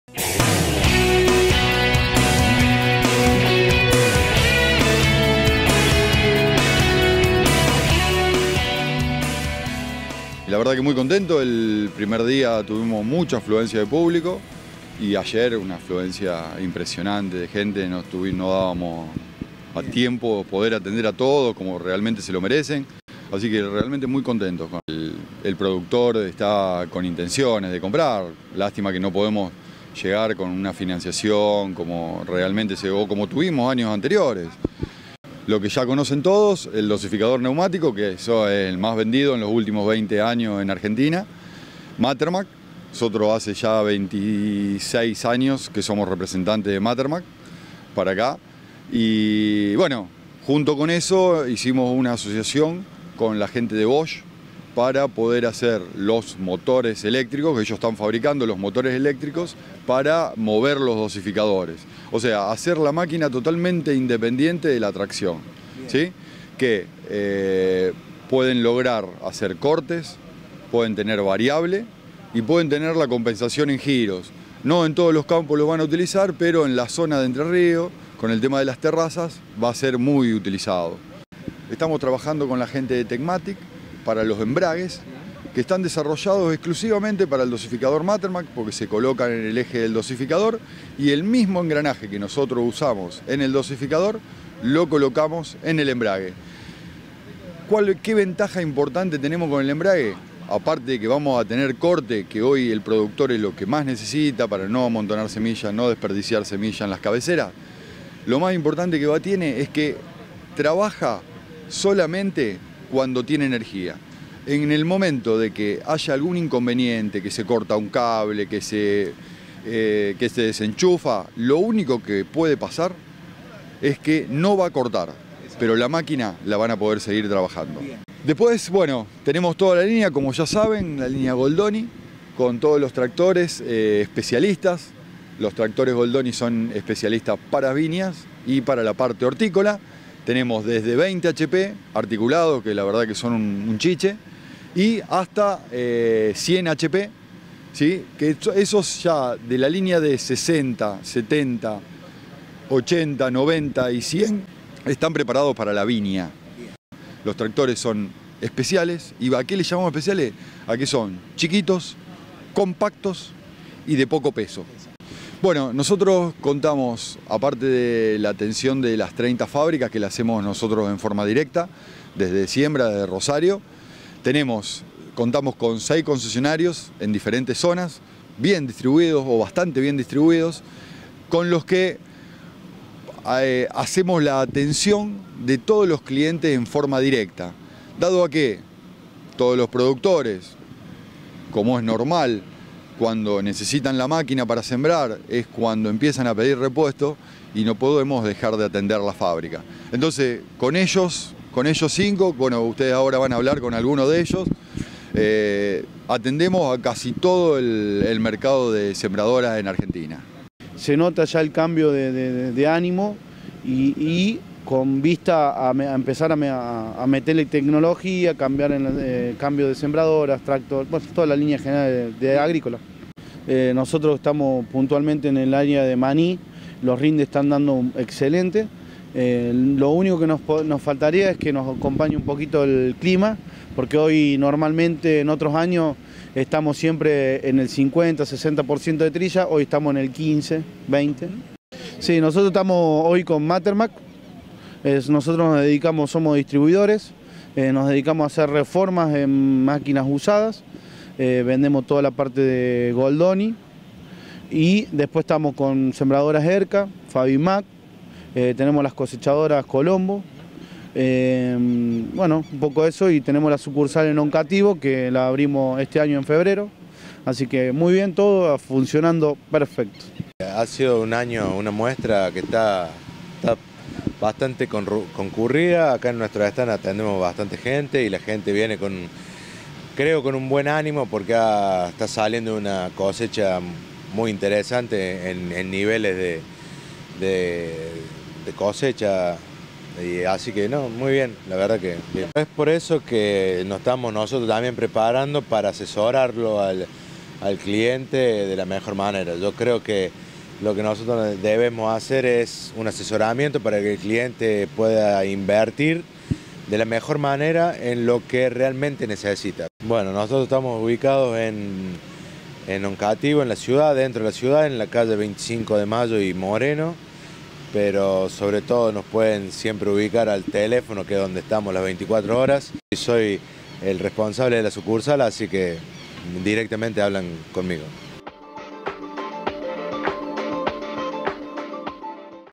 Siembra Neumática presente en Agroactiva 2019
Agroactiva, la mayor muestra del agro del país, marcó el camino del sector para los próximos años.